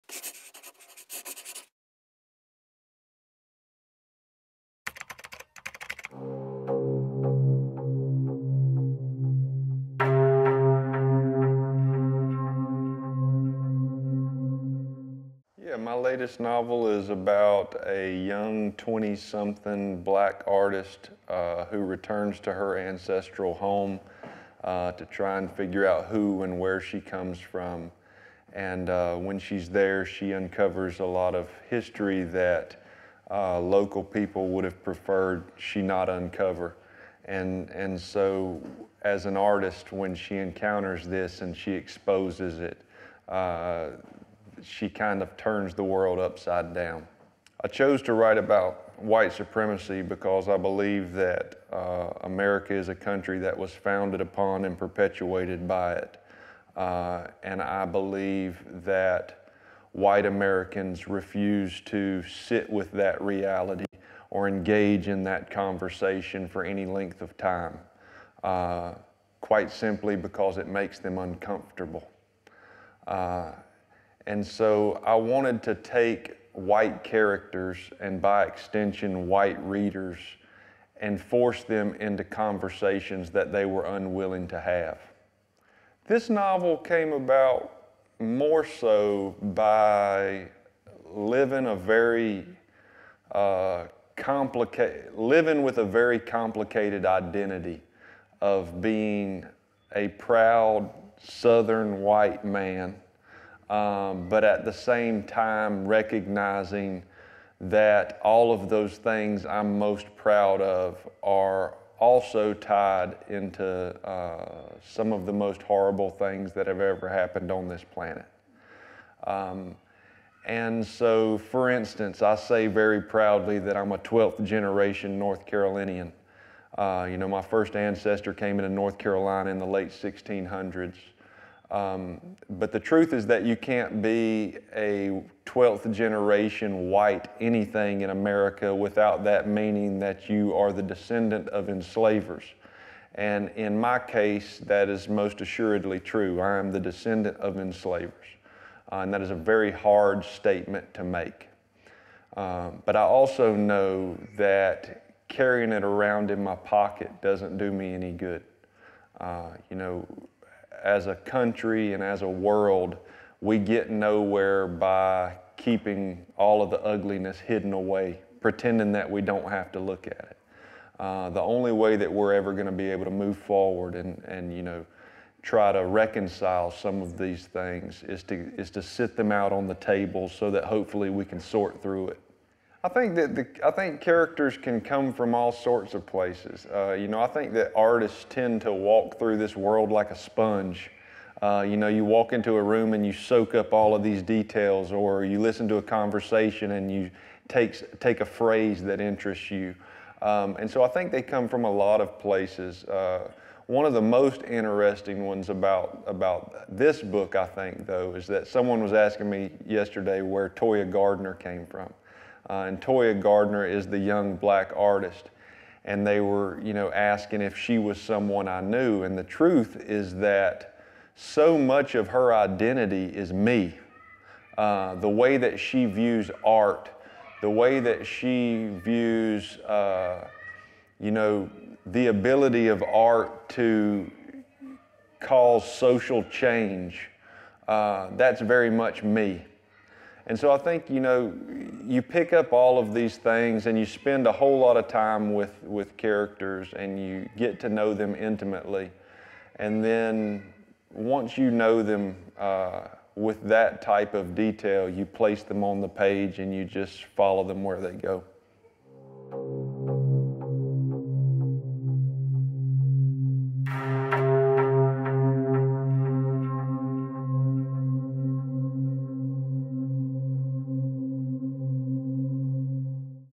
David Joy - Les deux visages du monde
À l'occasion du festival "America" qui s'est déroulé du 26 au 29 septembre 2024 à Vincennes, David Joy vous présente son ouvrage "Les deux visages du monde" aux éditions Sonatine. Rentrée littéraire automne 2024.